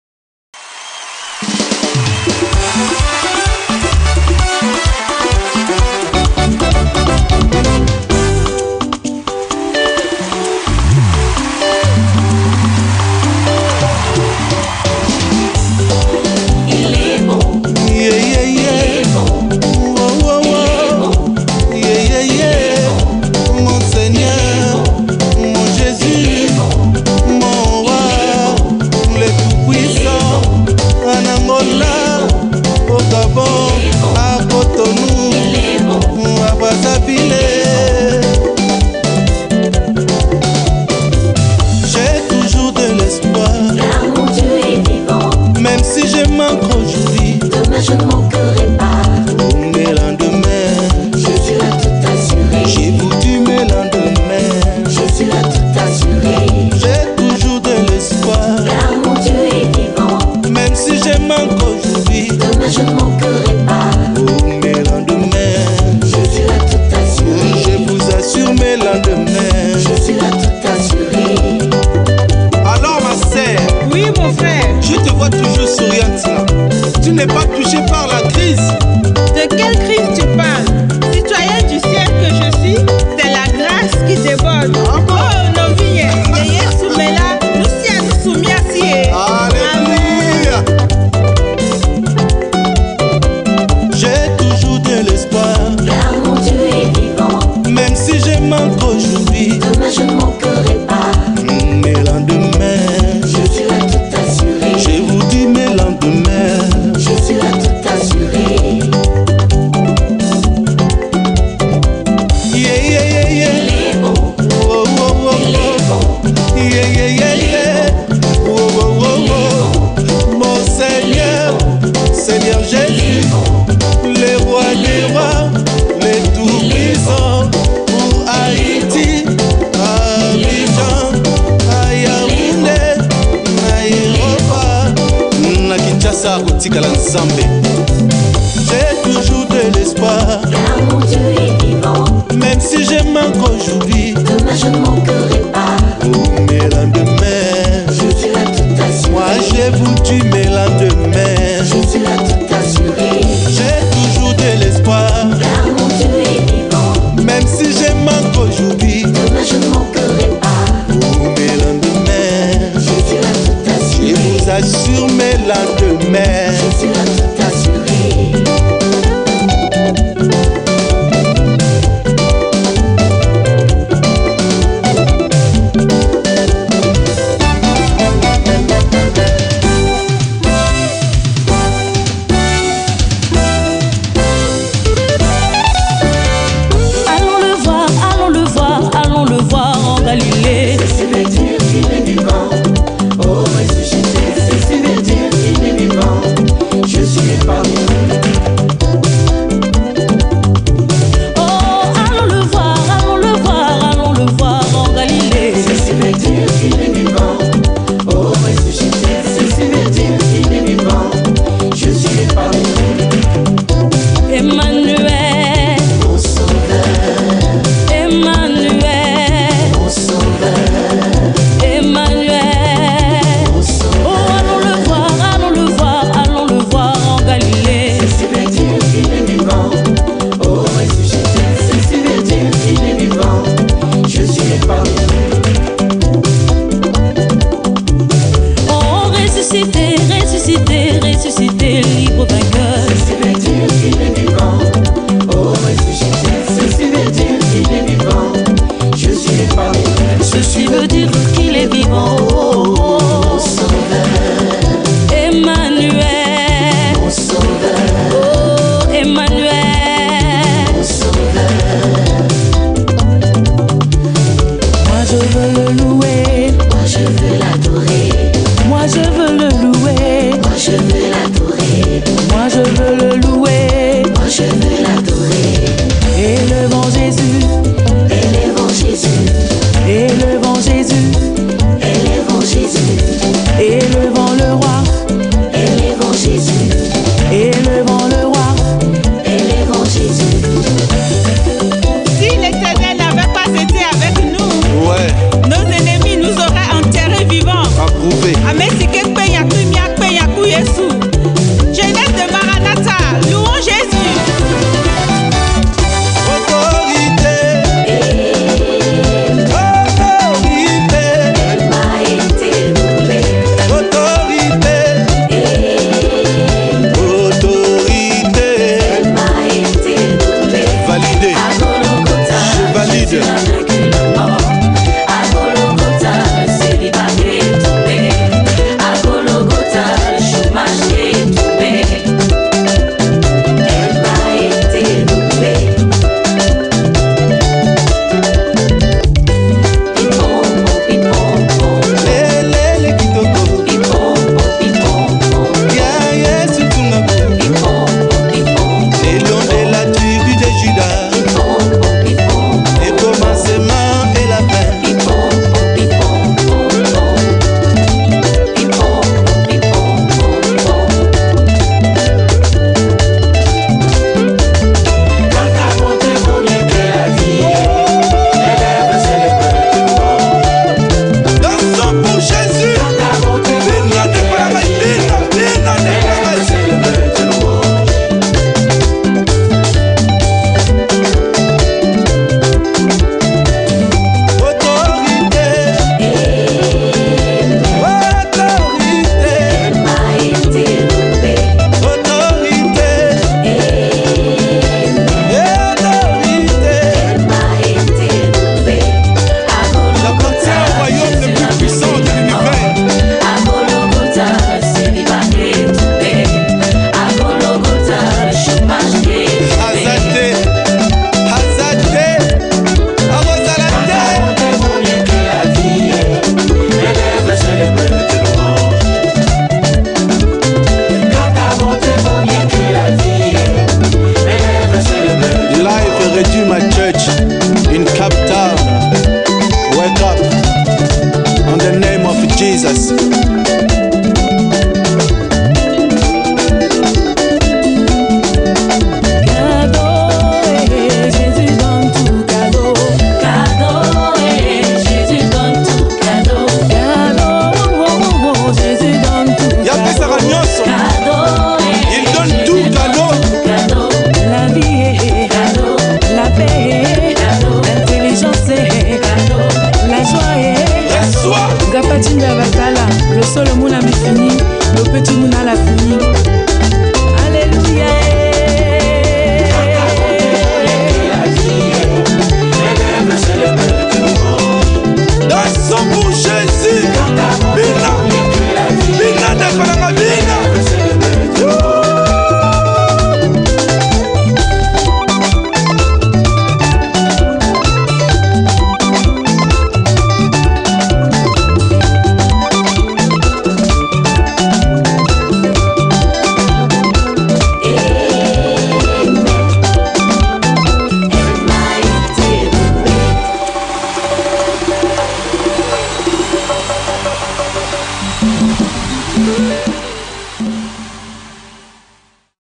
le clip du frère Nixon Mavinga.Regardez un journaliste danseR pour la gloire de Dieu!
Célébrons-le avec nos louanges et nos voix!